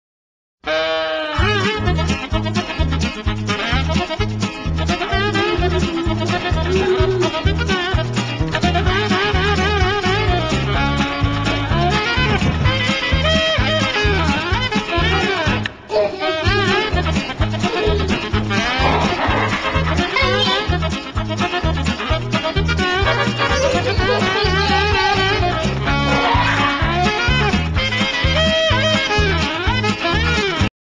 The Chase
Troy tries to run away from his mom.